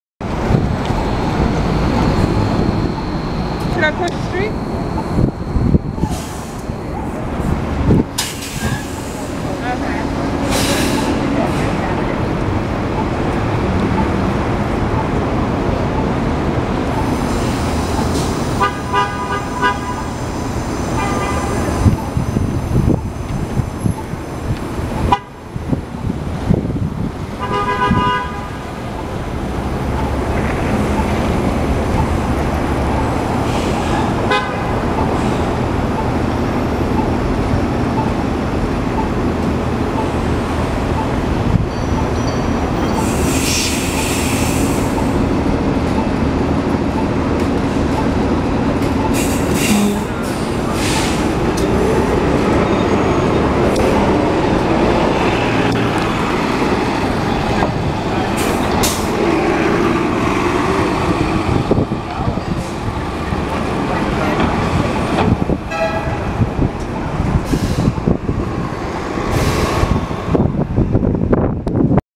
MTA NYC Bus 2022 New Flyer XD40 Xcelsior #7962 On The B41 @ Livingston Street & Smith Street
Here is 2022 New Flyer XD40 Xcelsior #7962 on the B41 making it's stop at Livingston Street and Smith Street, heading towards Tillary Street and Cadman Plaza.

Cummins L9 & Allison B400R